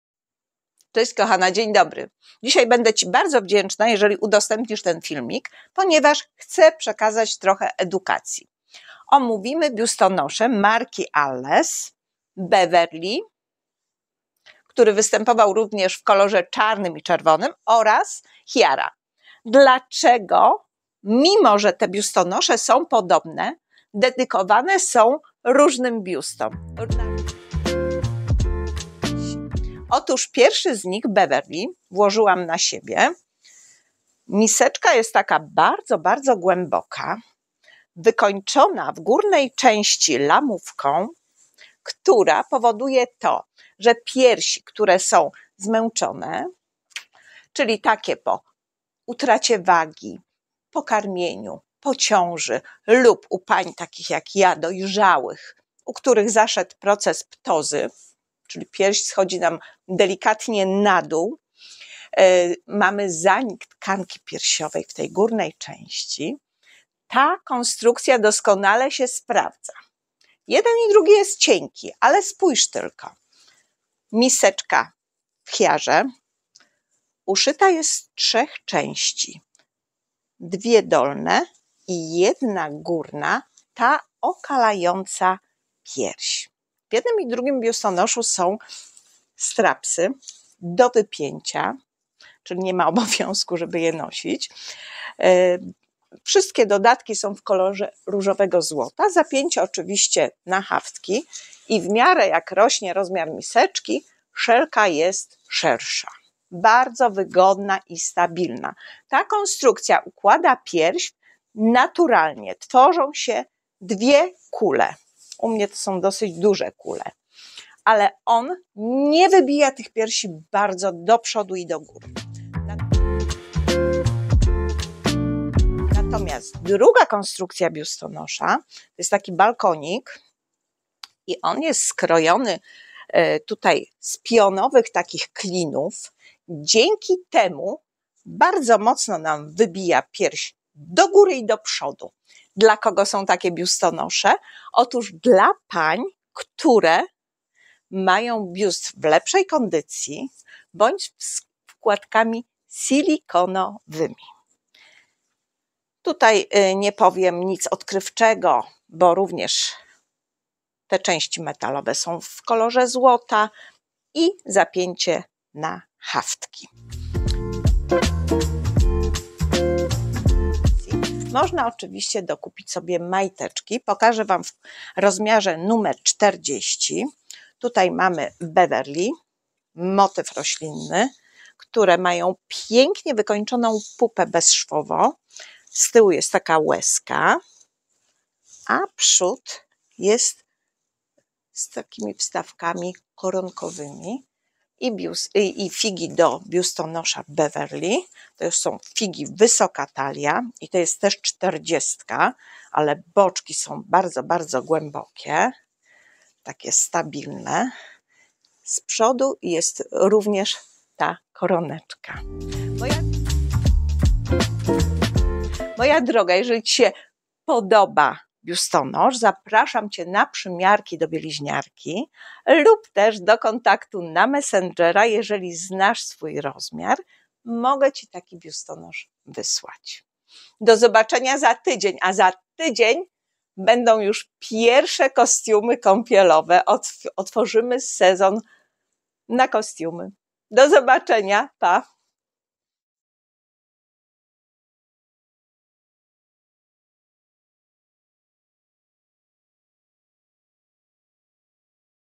Dwie propozycje walentynkowe, niby podobne, a jednak! Diabeł tkwi w szczegółach!
😈💓😈 Dwa biustonosze, jeden producent, zbliżony kolor, odpinane strapsy. Ale tylko jeden z nich byłby odpowiedni dla Ciebie. Chcesz wiedzieć dlaczego ⁉ Zapraszam na seans 🍿📽🍿

A to, że biustonosze w kolorze fuksji pojawiają się rzutem na taśmę przed Walentynkami, to oczywiście przypadek 😅

💓Najważniejsze informacje:

1️⃣ Biustonosz Alles  Beverly
▶️ Soft z odpinanymi strapsami (paseczkami)
▶️ Wykrojona balkonetka miękka idealna dla piersi z wkładkami silikonowymi lub wystarczająco jędrnych
▶️ Nowoczesna koronka z motywem  roślinnym
▶️ Rozmiary: 70 E, 75 C - E, 80 C - D, 85 C
▶️ Figi klasyczne do kompletu (36 - 42)

2️⃣ Biustonosz Alles Chiara
▶️ Zabudowana z wyciągniętą miseczką ku górze idealna dla piersi zmęczonych, po karmieniu, zmianie wagi lub kobiet dojrzałych
▶️ Klasyczna koronka z motywem kwiatów
▶️ Rozmiary: 75 F - H, 80 D - H, 85 D - H, 90 D - F, 95 D - E
▶️ Figi wysoki stan do kompletu (38 - 46)

Serdecznie zapraszam na przymiarki 🤗
              👉 Bieliźniarka
                    Głubczyce
                    Kochanowskiego 6
PS. Jeśli interesuje Cię wysyłka biustonosza zapraszam na priv (wysyłka PL 15zł) 🥰

00:00 Wstęp
00:30 Alles Chiara Fuksja
02:06 Alles Beverly Fuksja
02:51 Figi do kompletu
03:35 Dziękuję za uwagę!